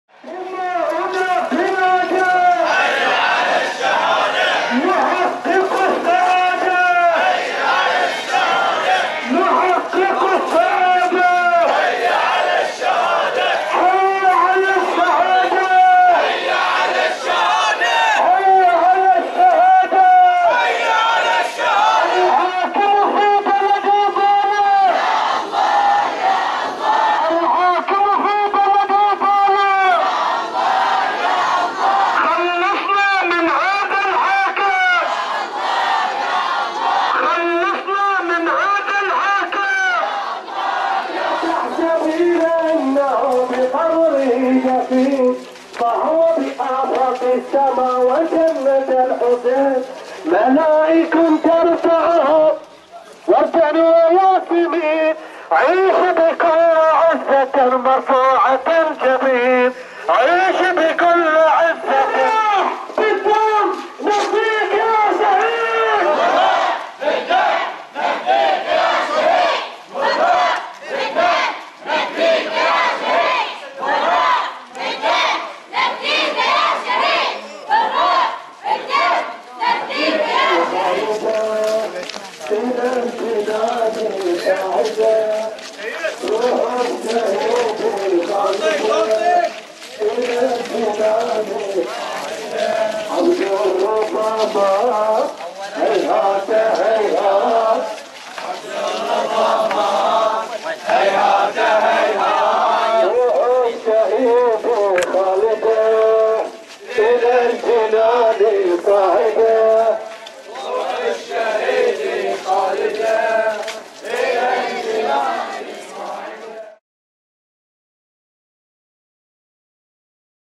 المالكية | التشييع الرمزي للشهيد فاضل العبيدي 10/3/2012
Malkiya Voice | صوت المالكية